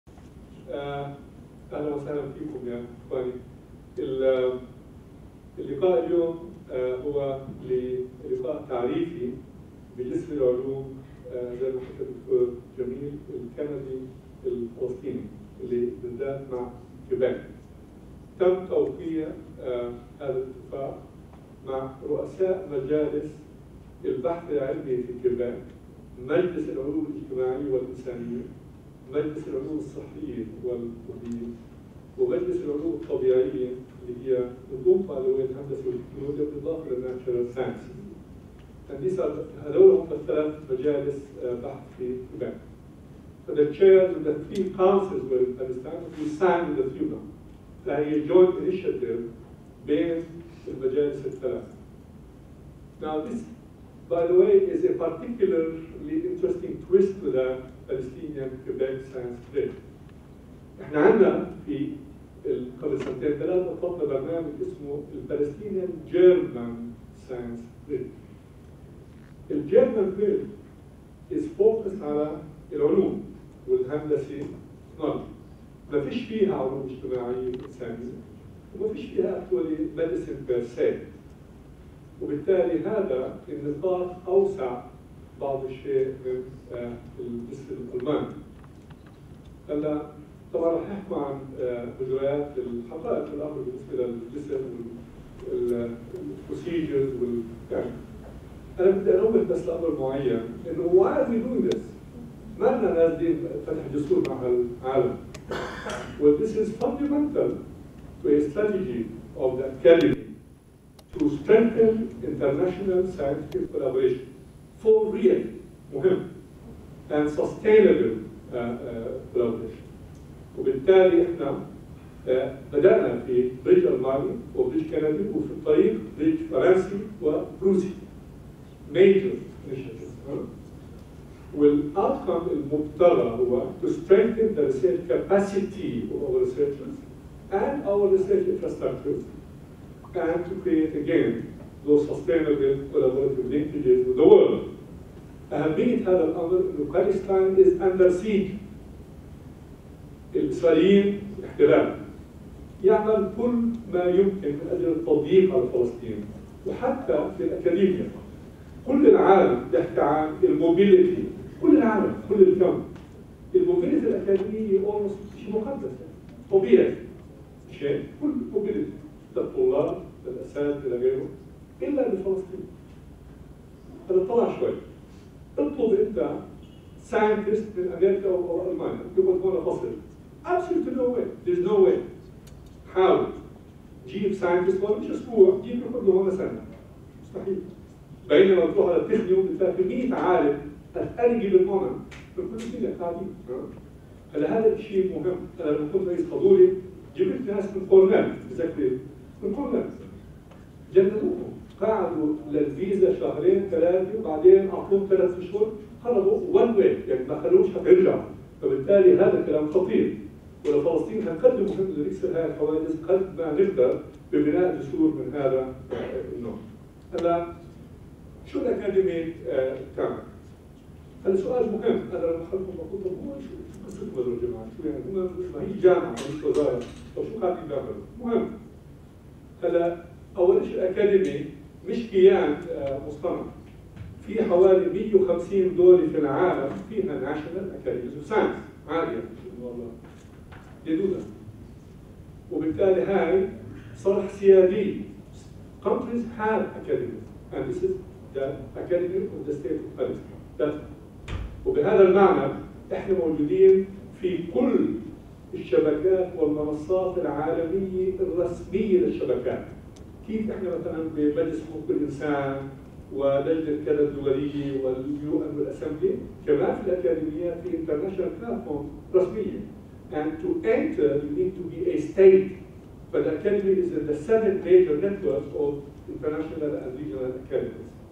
Dr. Marwan Awartani's opening remark
The Palestine Academy for Science and Technology held an information day within the Palestinian Quebecer Science Bridge (PQSB) and announced the launch of the second call, as part of the mission to enhance the exchange of Palestinian and Quebecer Researchers.
PQSB aims to support short-term scientific missions for Palestinian researchers whose research has already produced notable results in any of the fields of health sciences, natural sciences and engineering, or social sciences and humanities, arts and letters. The main and long term objective of Palestine-Quebec Science Bridge is to foster collaboration between researchers in Quebec and their counterparts in Palestine (especially early-career researchers) through staff/ faculty exchange, and other mobility programs.

These programs will provide an opportunity to integrate Palestinian researchers into FRQ-funded research projects and its network of national and international partners and their research funding programs which will allow them to carry out their scientific research projects in collaboration with researchers in Quebec.

The first call for the PQBS was launched in January 2018,  where researchers were selected from 13 Palestinian Higher Education Institutions, and the selection process was transparent and based on merit.